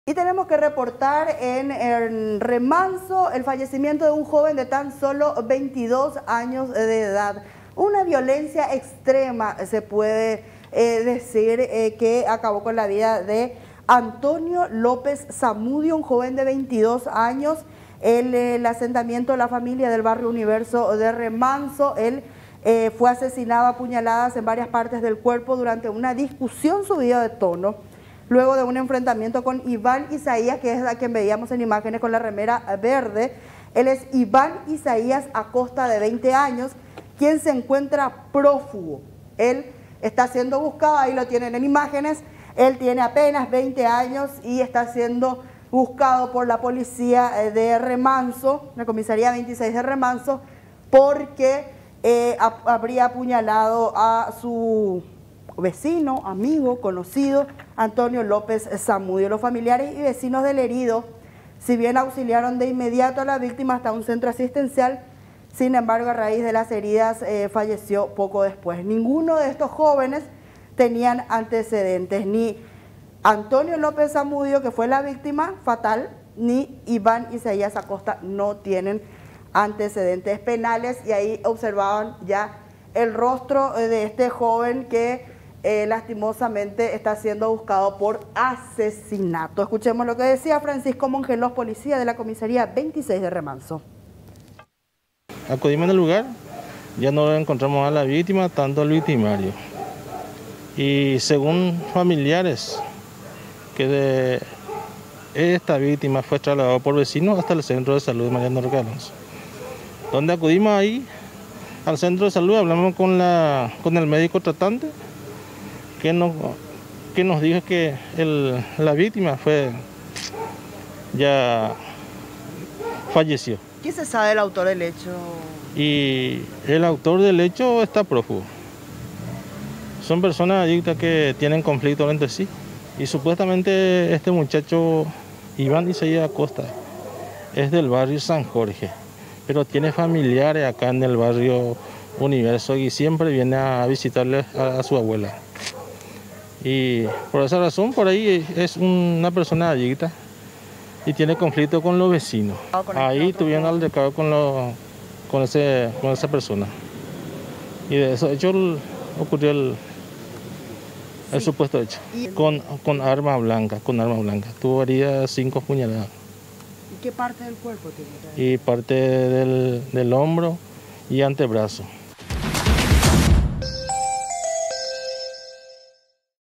Asesinan de cinco puñaladas a un joven en Remanso
En el asentamiento La Familia del Barrio Universo de Remanso, Antonio López Samudio, un joven de 22 años, fue asesinado a puñaladas en varias partes del cuerpo durante una discusión subida de tono y luego un enfrentamiento con Iván Isaías Acosta de 20 años, quien se encuentra prófugo.